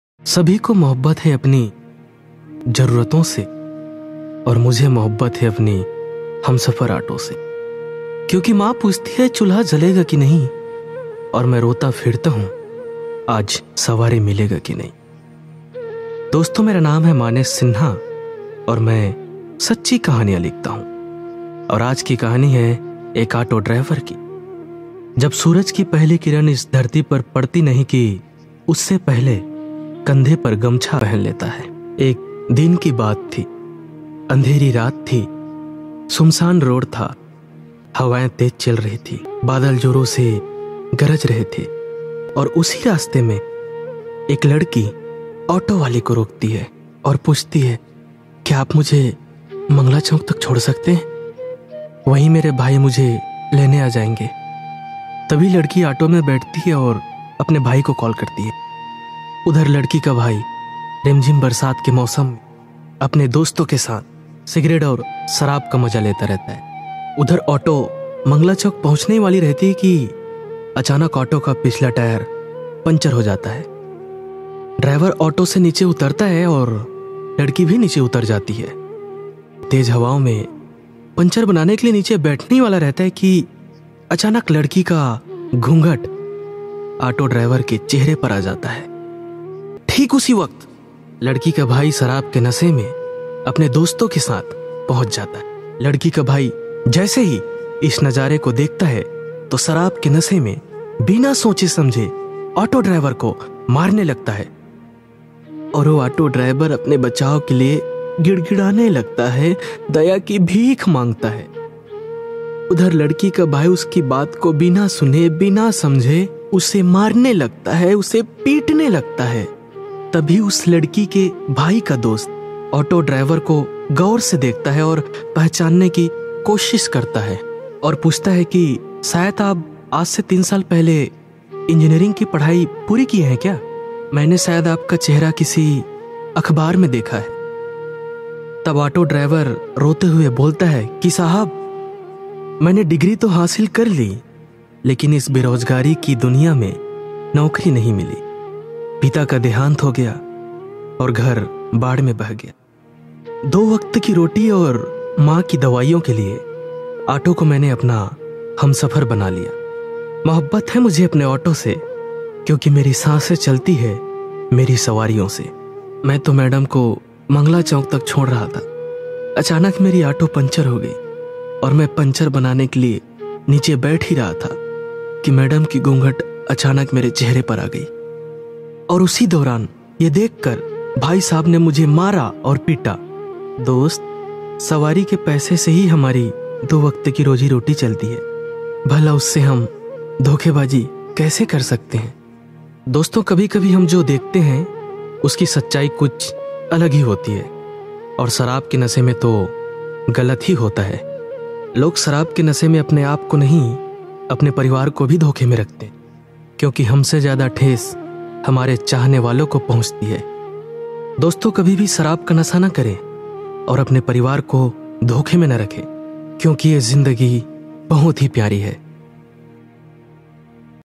Garib Auto Driver। गरीबऑटो ड्राइवर।Manesh Sinha। Motivational Video
दोस्तों ये  कहानी एक गरीब ऑटो ड्राइवर की है जो अपनी गरीबी के कारण डिग्री हासिल करके भी ऑटो चलाता है दोस्तो शराब एक ऐसा नशा है जो लोगो को बिगाड़ने में समय नहीं लगता ये वीडियो एक संदेश देती है कि नशे में हम सही गलत की पहचान नहीं कर पाते और खुद को धोखे में रखते है ।।।

Cast- Surya Nevendra, Manesh Sinha, Suman Mahant, Sandeep, Priyanshu Rajput, Yashvant
Recording- Swar Music Academy Bhilai
Production Team - Amanjeet Mahant, Surya Nevendra, Vinod kumar, priyanshu Rajput
Supporting - Naveen Chhinta(HP)
Poster - Limitless graphics
Special Thanks- Anand manikpuri, Tushar manikpuri